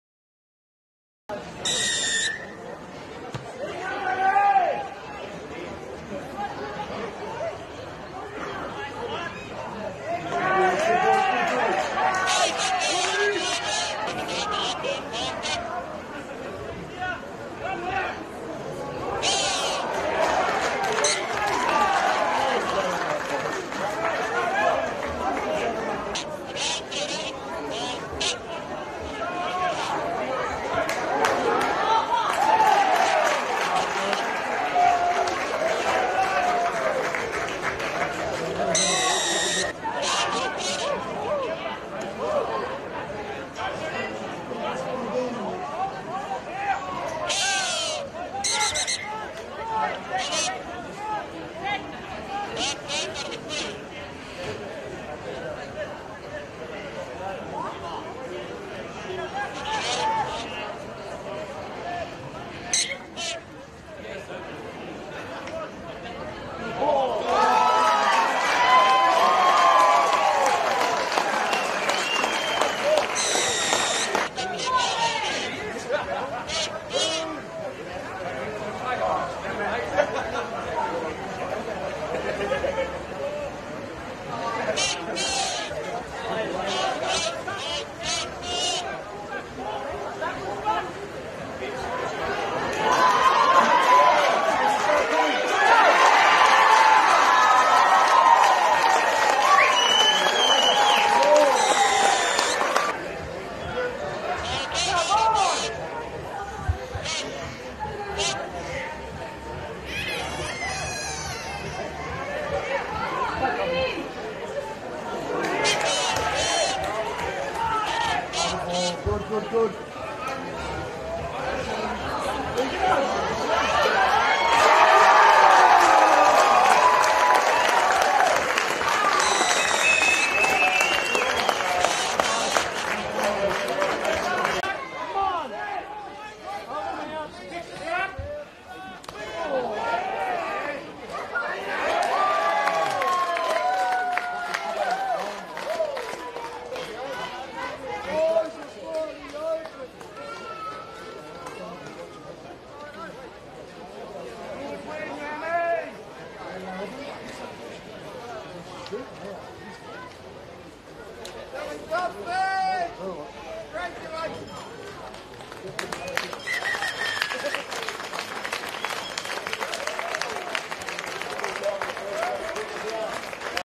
Woden Valley Rams v Queanbeyan Blues - Round 1 Highlights | Canberra Region Rugby League 2022
2022 Canberra Region Rugby League
First Grade Round 1 Highlights
Woden Valley Rams v Queanbeyan Blues